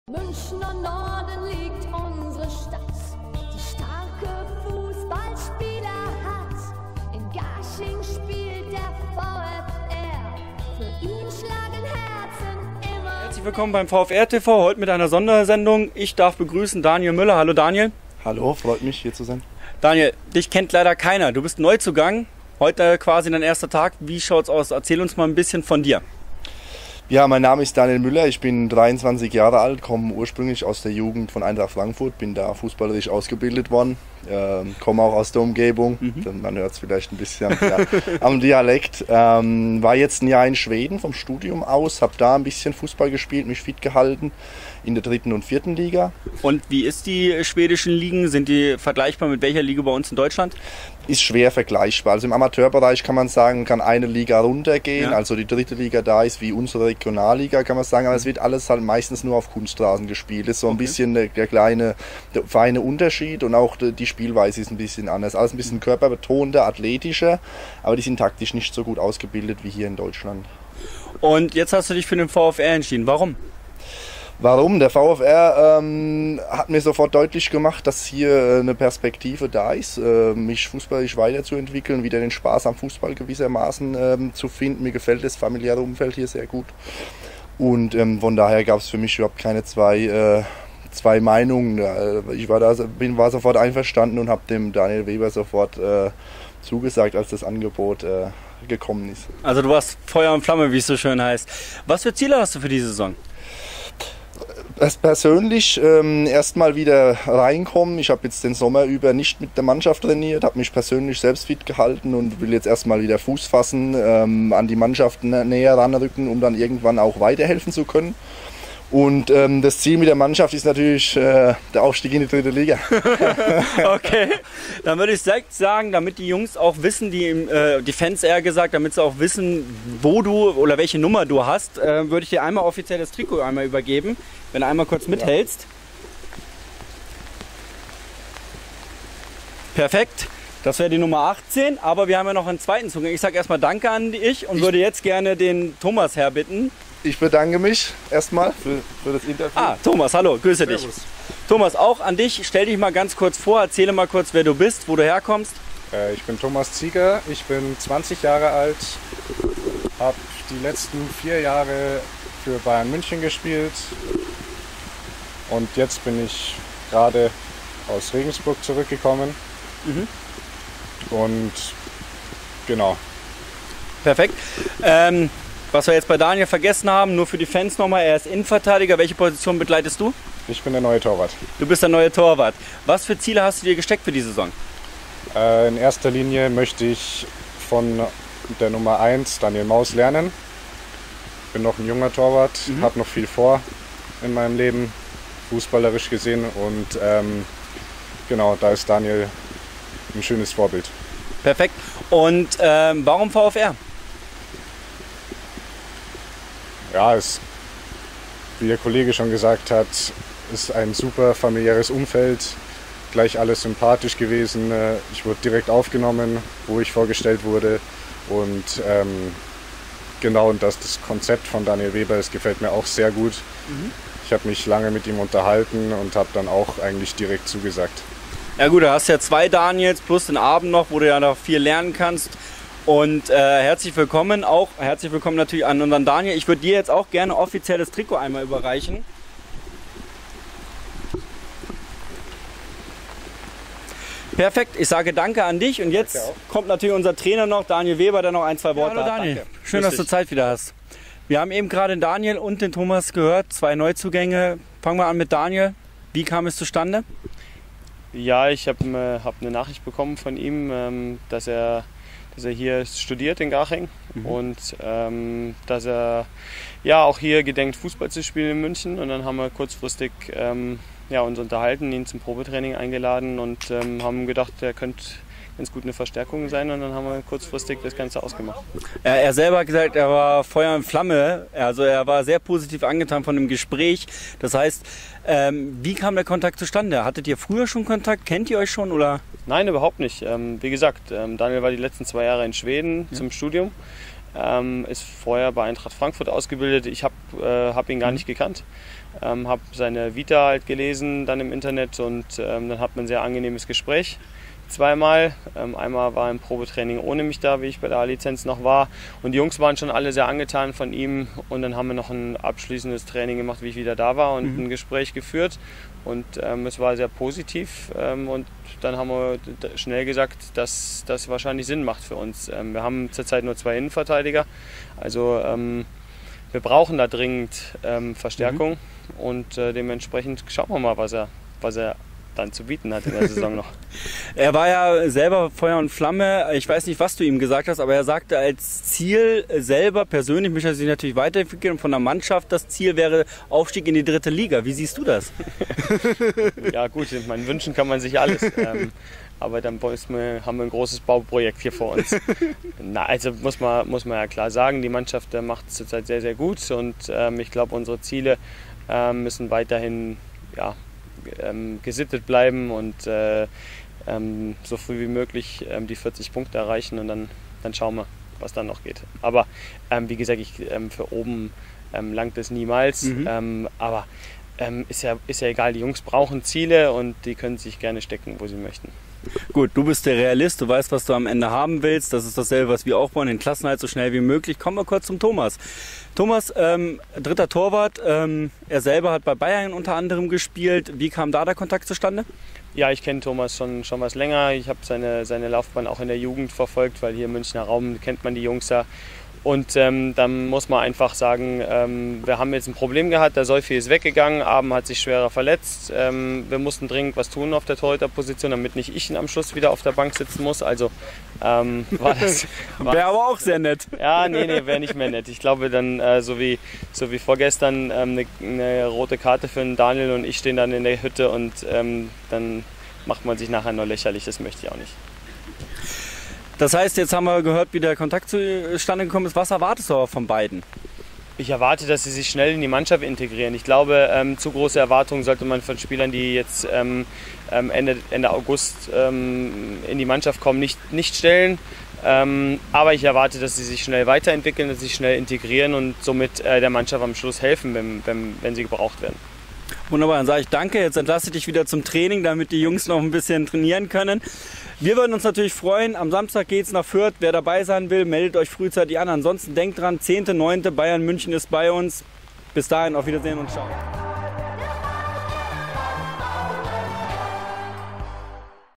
Neuzugänge stellen sich vor
Daniel Müller und Thomas Zieger stellen sich vor. Des Weiteren hören wir unseren Trainer Daniel Weber